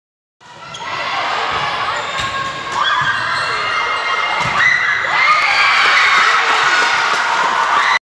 2020 Club Volleyball Power 3 @ Elevation
Video clips from Power 3 tournament at Elevations VBC in Colorado featuring Five Star 17 Black Volleyball Club Team